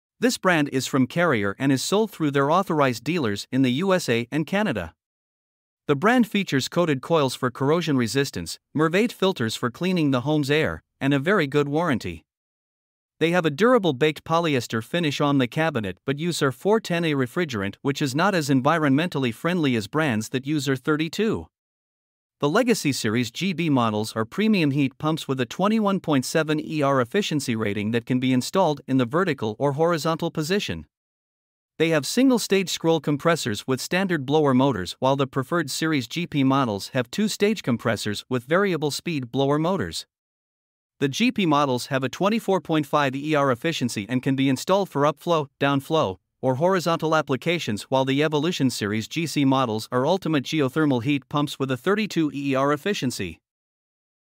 HvacRepairGuy 2023 Bryant Brand Geothermal Heat Pump Reviews
In this video, HvacRepairGuy reviews the 2023 Bryant brand of geothermal heat pump units. The reviews include the premium Legacy Series GB & Preferred Series GP units, as well as the ultimate Evolution Series GC models.
You can get the reviews of all of the brands